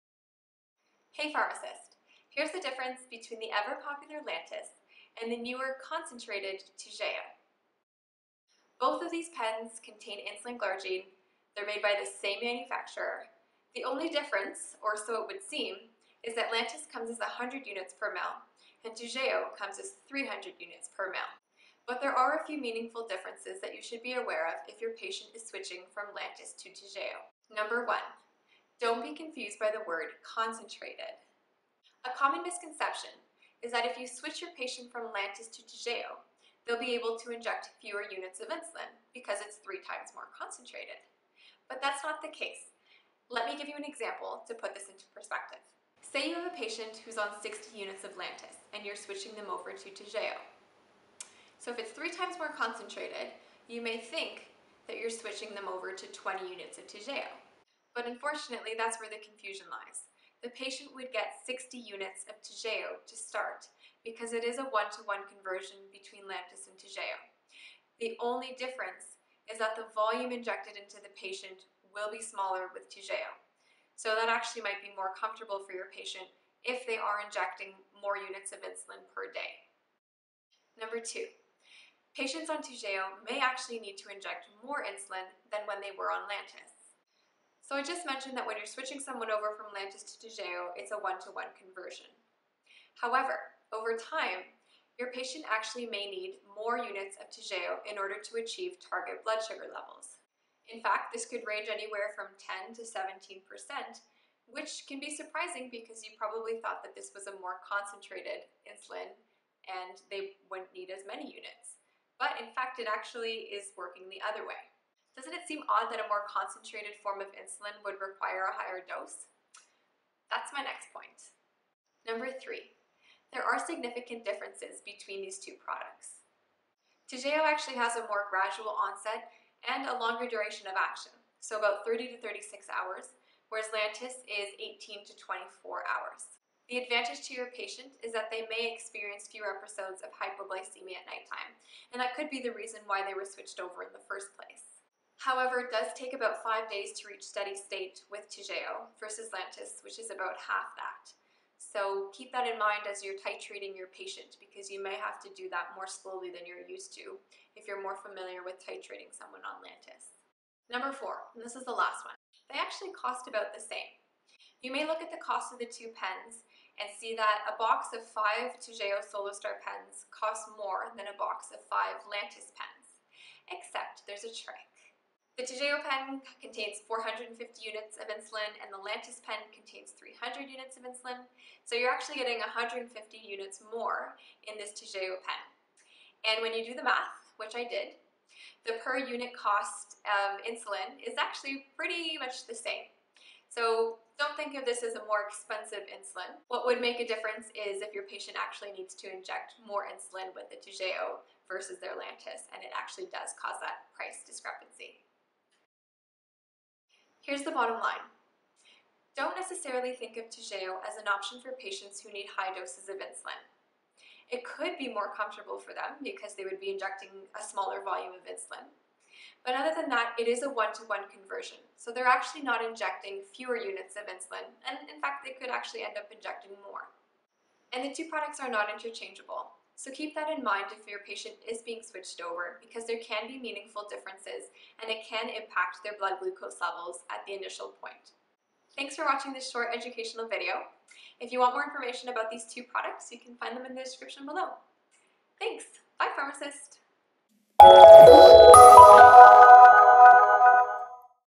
Lantus vs. Toujeo: What's the difference?
Toujeo vs. Lantus - What Pharmacists Need to Know.

References:
Toujeo - products.sanofi.ca/en/toujeo-solostar.pdf
Lantus - products.sanofi.ca/en/lantus.pdf

I hope you learned something today!